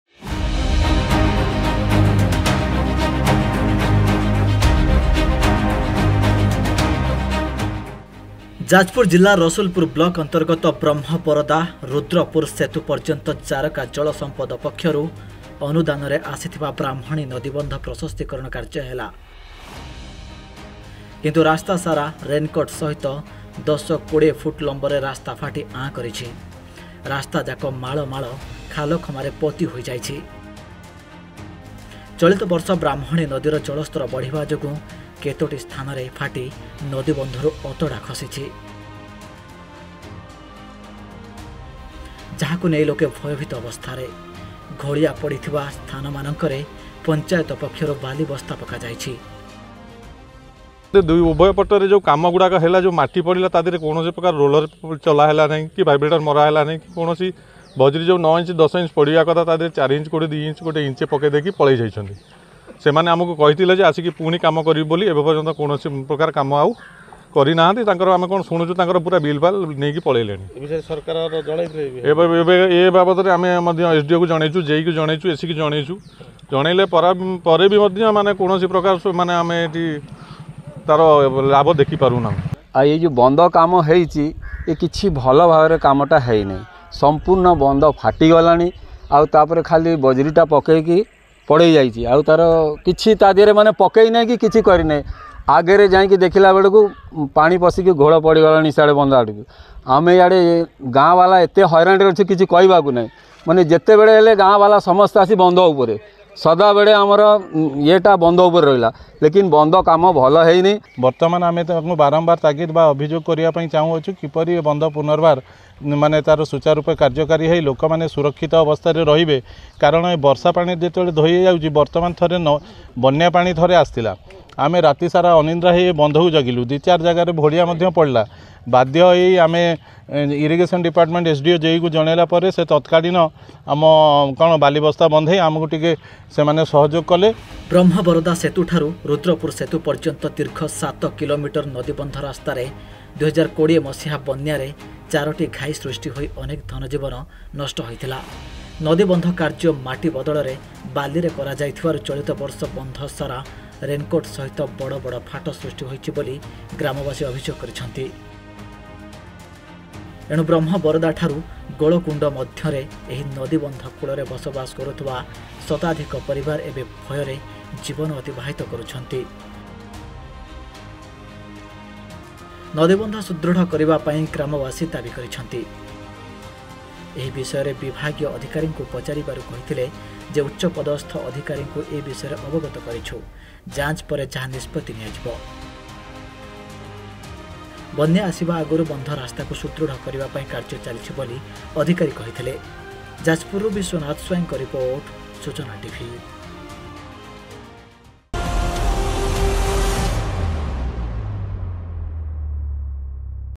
ନଦୀବନ୍ଧ ପ୍ରଶସ୍ତିକରଣ ସରିଥିଲେ ସୁଦ୍ଧା ଚାରିଆଡେ ମାଳମାଳ ଖାଲ, ଡରୁଛନ୍ତି ଲୋକମାନେ
ଯାଜପୁର ଜିଲ୍ଲା ରସୁଲପୁର ବ୍ଲକ ଅନ୍ତର୍ଗତ ବ୍ରହ୍ମବରଦା ରୁଦ୍ରପୁର ସେତୁ ପଯ୍ୟନ୍ତ ଜାରକା ଜଳ ସମ୍ପଦ ପକ୍ଷରୁ ଅନୁଦାନରେ ଆସିଥିବା ବ୍ରାହ୍ମଣୀ ନଦୀବନ୍ଧ ପ୍ରଶସ୍ତିକରଣ କାର୍ଯ୍ୟ ହେଲା । କିନ୍ତୁ ରାସ୍ତା ସାରା ରେନ୍ କଟ ସହିତ 10/20 ଫୁଟ ଲମ୍ବରେ ରାସ୍ତା ଫାଟି ଆଁ କରିଛି । ରାସ୍ତା ଯାକ ମାଳ ମାଳ ଖାଲ ଖମାରେ ପୋତି ହୋଇଯାଇଛି । ଚଳିତ ବର୍ଷ ବ୍ରାହ୍ମଣୀ ନଦୀର ଜଳସ୍ତର ବଢିବା ଯୋଗୁଁ କେତୋଟି ସ୍ଥାନରେ ଫାଟି ନଦୀ ବନ୍ଦରୁ ଅତଡ଼ା ଖସିଛି । ଯାହାକୁ ନେଇ ଲୋକେ ଭୟଭୀୟ ଅବସ୍ଥାରେ ।  ଘୋଳିଆ ପଡିଥିବା ସ୍ଥାନ ମାନଙ୍କରେ ପଂଚାୟତ ପକ୍ଷରୁ ବାଲି ବସ୍ତା ପକାଯାଇଛି ।

Dear Viewers,

Item ID:                         UBWNYFL
Author Username:                 marcobellonimusic
Licensee:                        suchana tv
Registered Project Name:         suchanatv24x7
License Date:                    April 30th, 2023